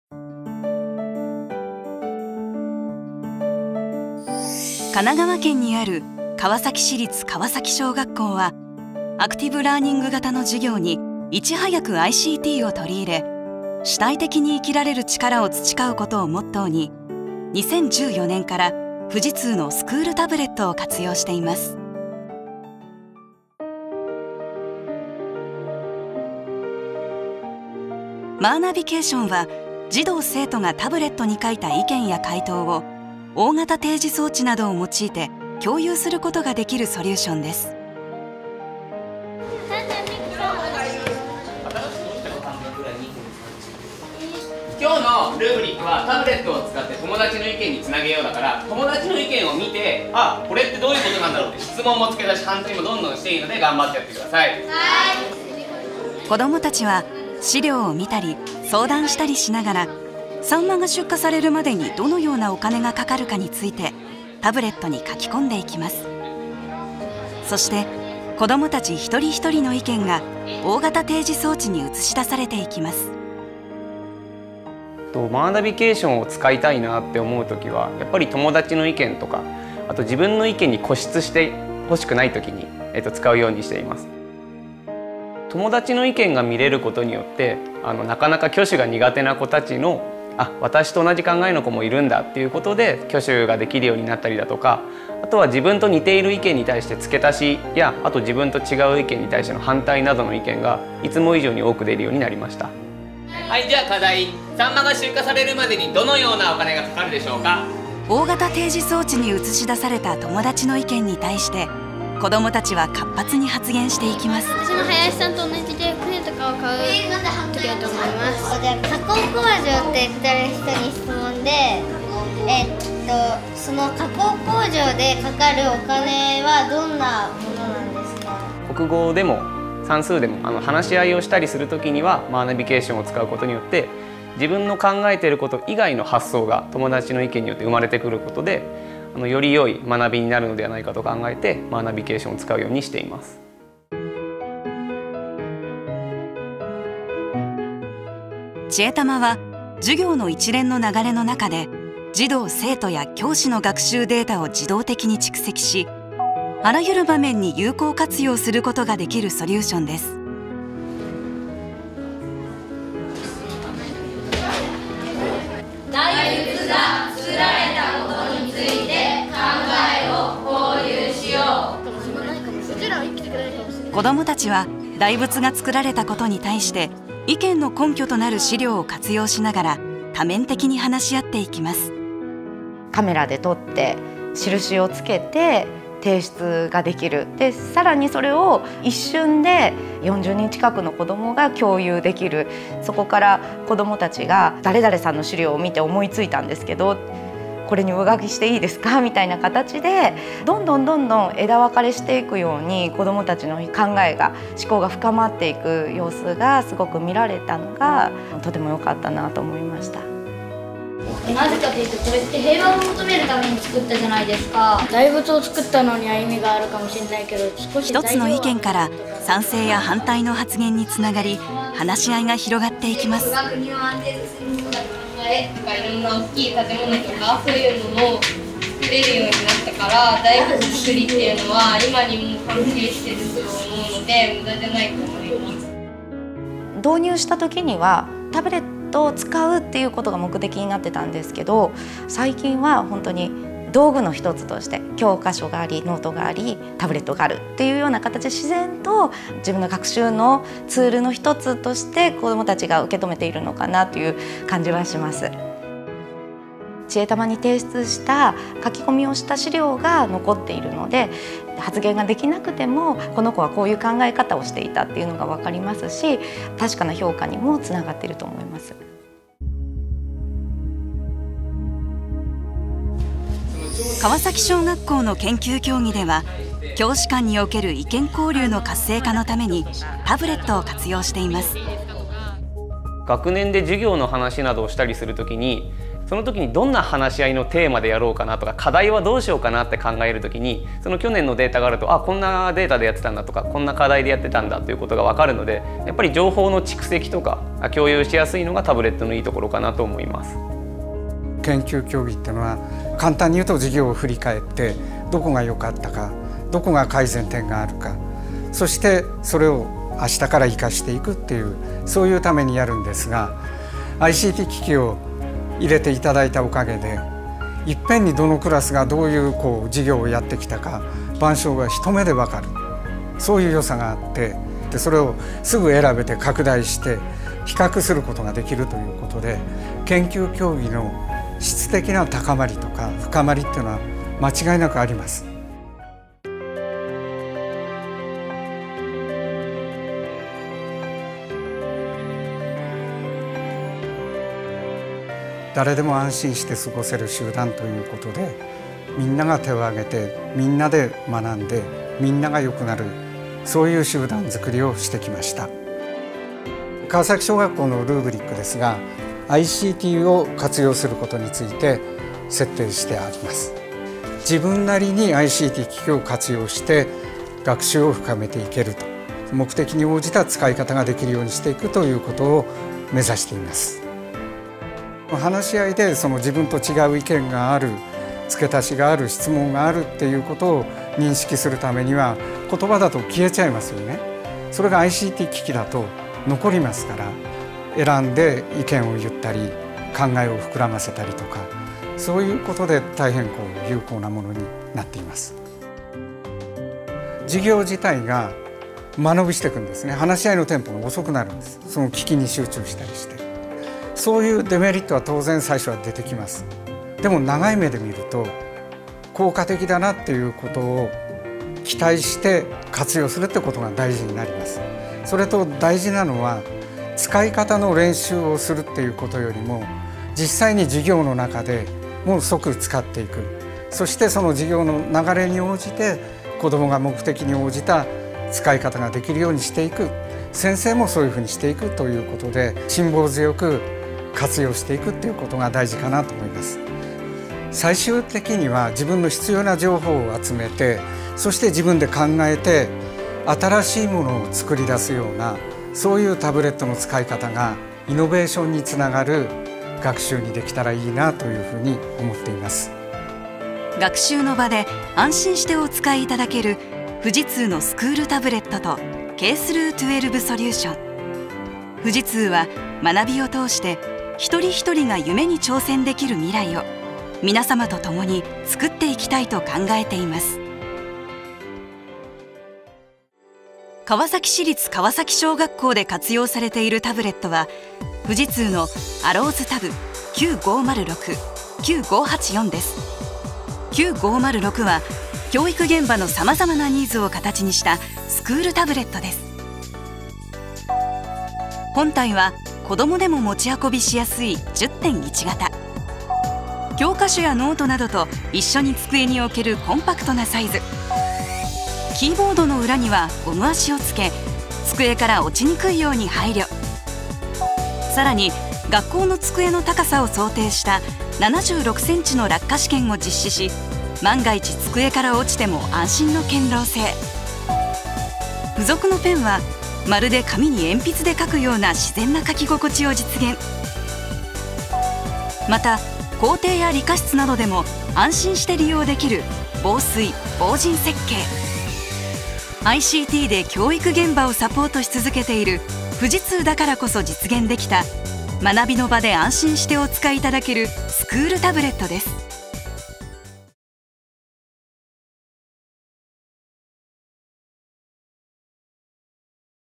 Fujitsu Education Solutions Case Study
See how Kawasaki Elementary School in Japan is harnessing ICT to prepare students for the future.